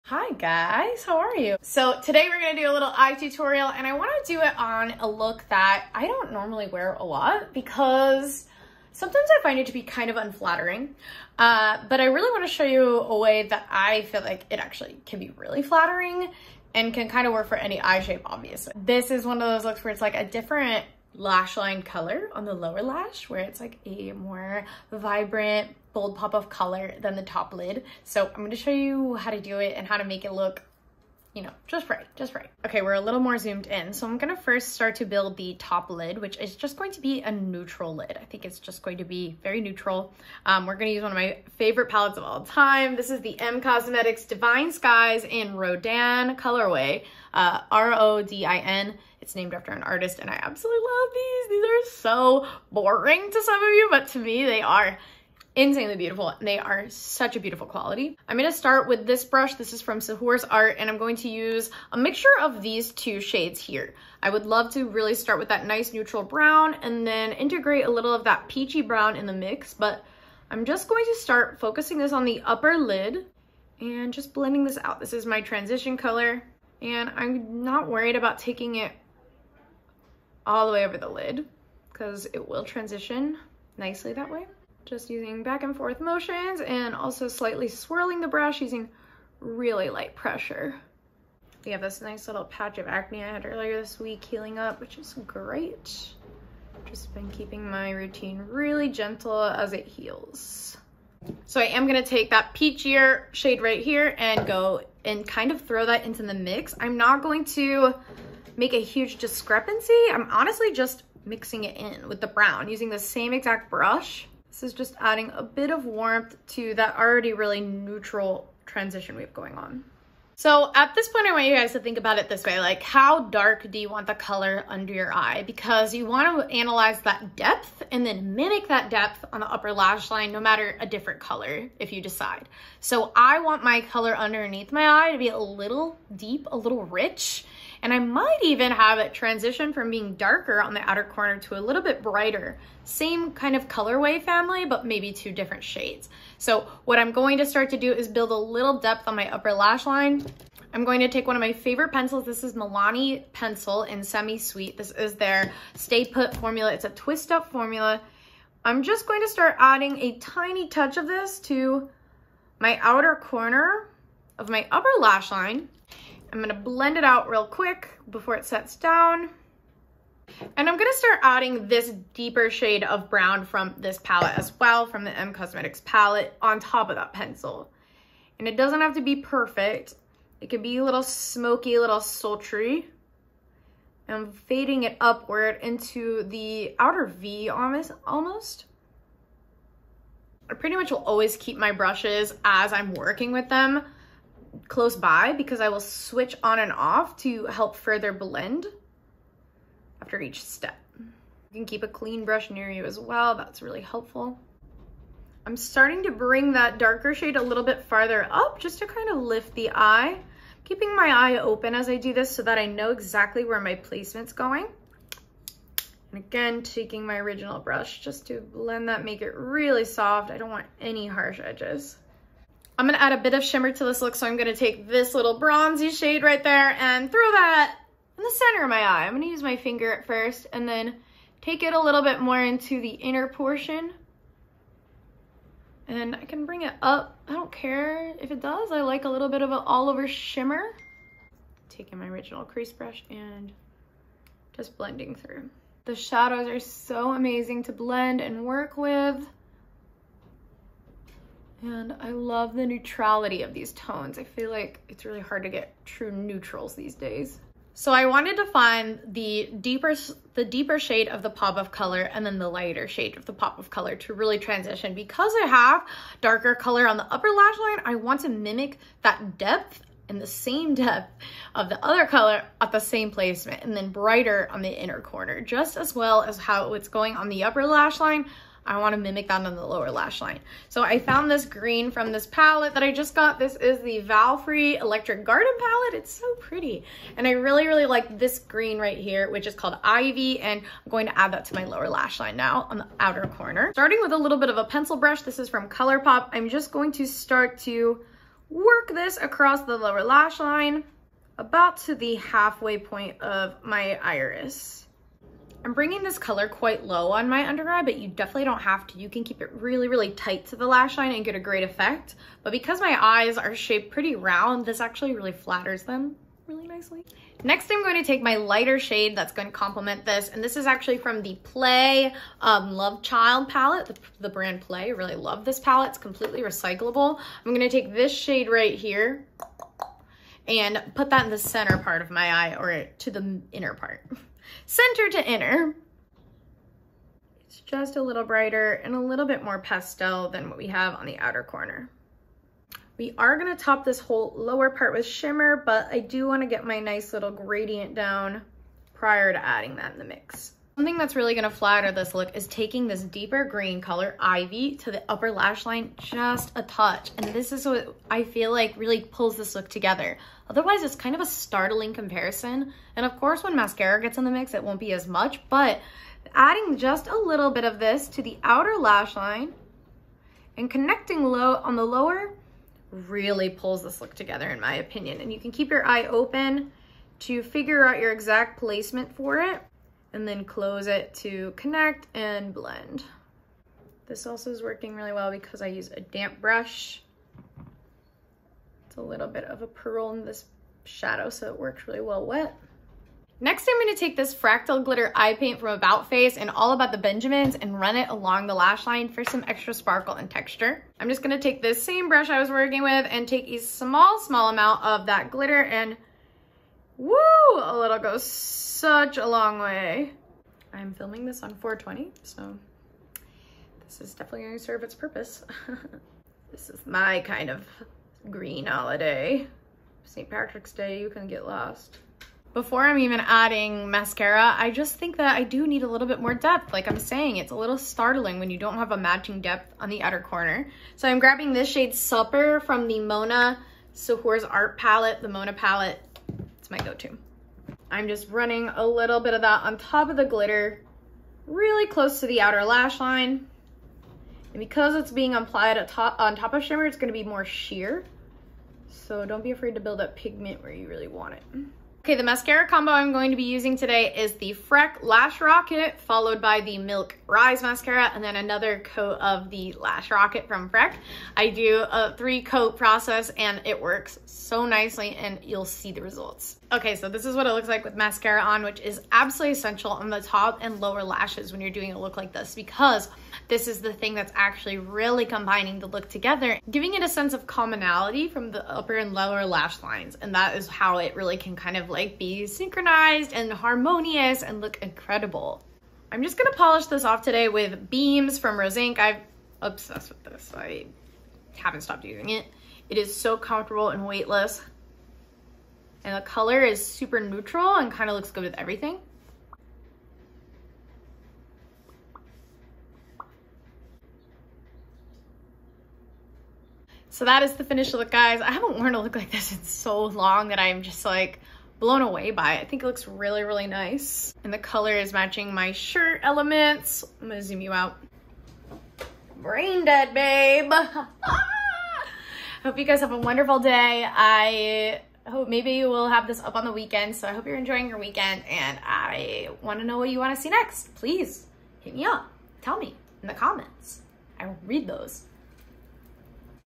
I never wear this style of look and maybe I never will again
I love this type of look on literally everyone besides myself, so I decided to try it again using some of my favorite products. We’ll see if I end up wearing it again.